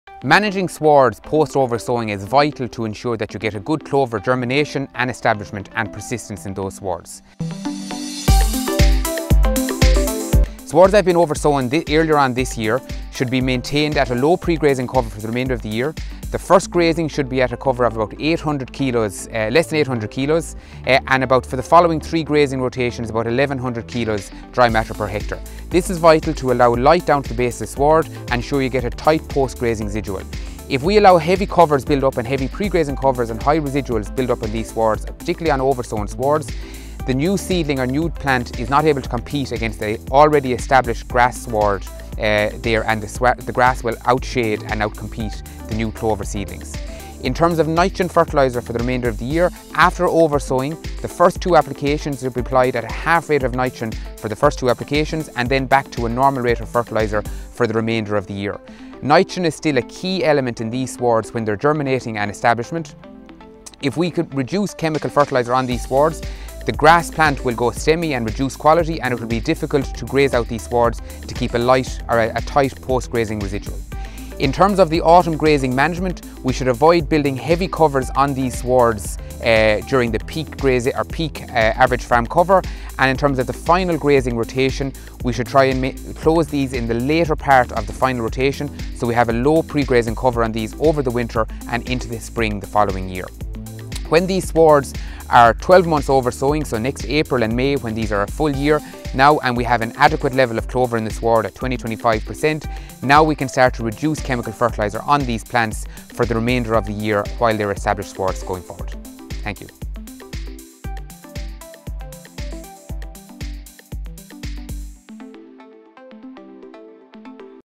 Managing Oversown Clover with Mike Egan
Oversown clover needs careful management this time of the year to ensure good establishment and persistency.  Mike Egan, Teagasc researcher at Moorepark provides an update on how to manage oversown clover at this time of year.